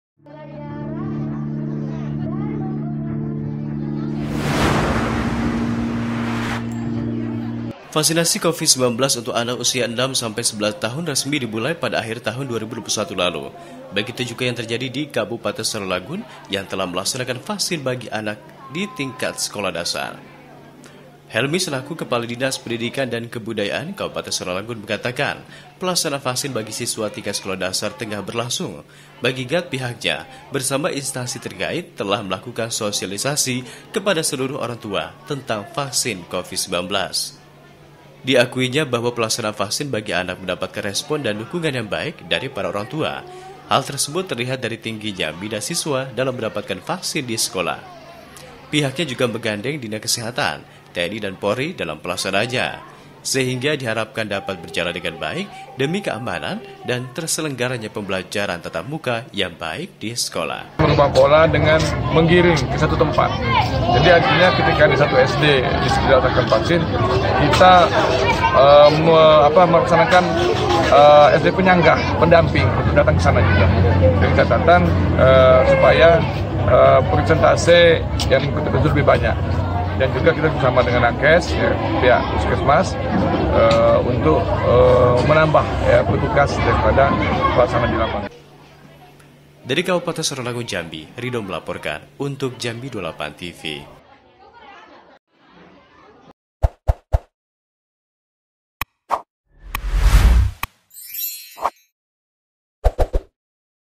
Vaksin COVID 19 Anak Sekolah Dasar di Sarolangun Terus Berjalan
JAMBI28TV, SAROLANGUN - Vaksinasi Covid-19 untuk anak usia 6 sampai 11 tahun resmi dimulai pada akhir tahun 2021 lalu, begitu juga yang terjadi di kabupaten Sarolangun yang telah melaksanakan vaksin bagi anak terima di tingkat sekolah dasar.

Helmi selaku kepala dinas pendidikan dan kebudayaan kabupaten Sarolangun mengatakan pelaksanaan vaksin bagi siswa tingkat sekolah dasar tengah berlangsung mengingat pihaknya bersama instansi terkait telah melakukan sosialisasi kepada seluruh orang tua tentang vaksin tersebut.